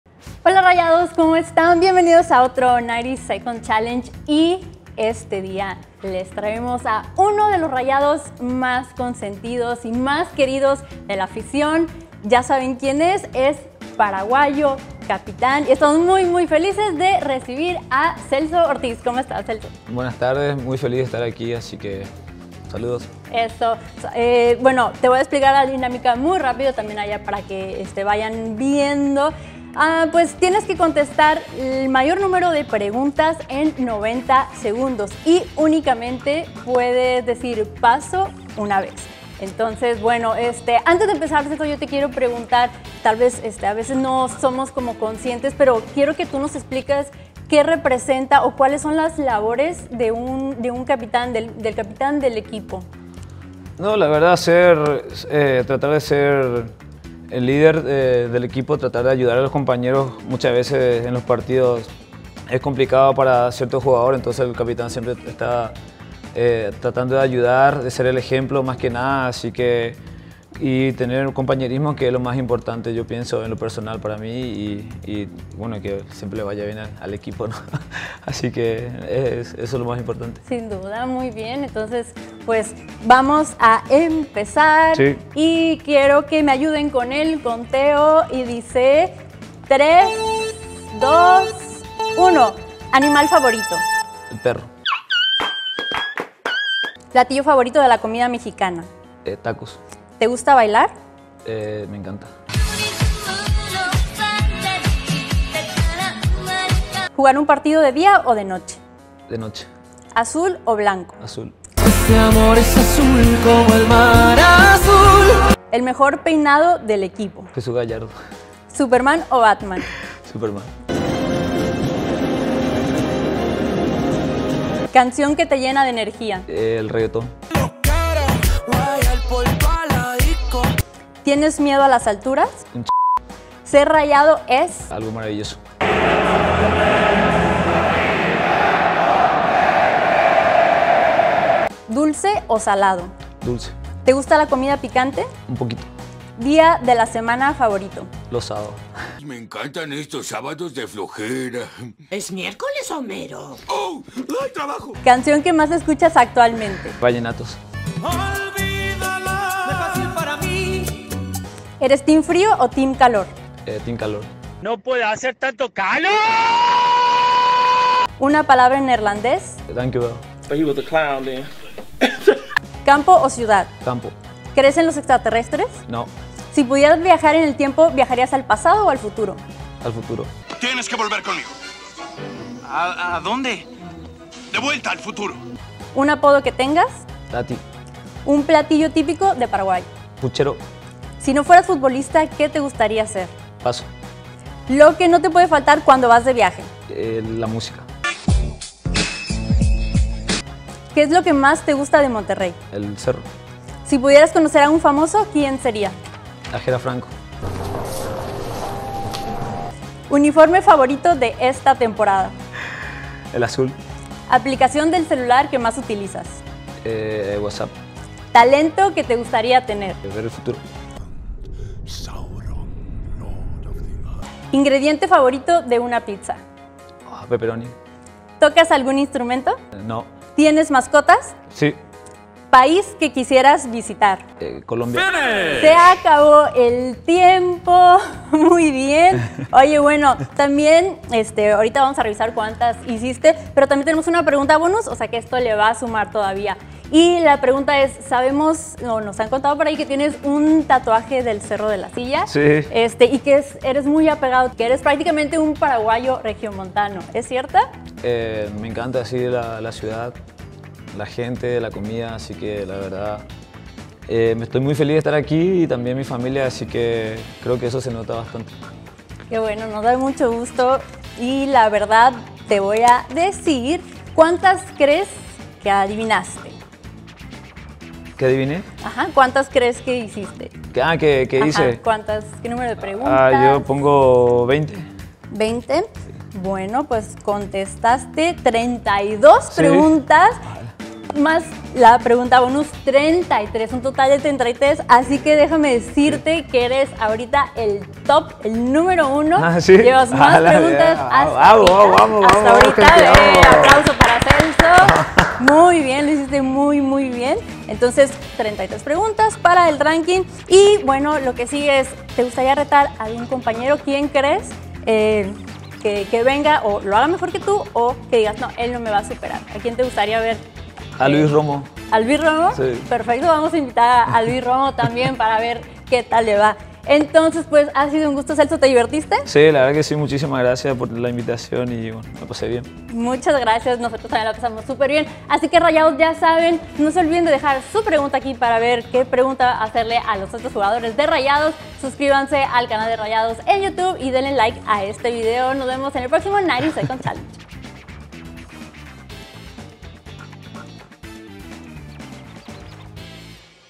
90SecondsChallenge con Valo Calles y Celso Ortiz
Un nuevo contendiente en el 90 Seconds Challenge llega y va con todo por el primer lugar. Aquí te dejamos el reto con Valo Calles y Celso Ortiz.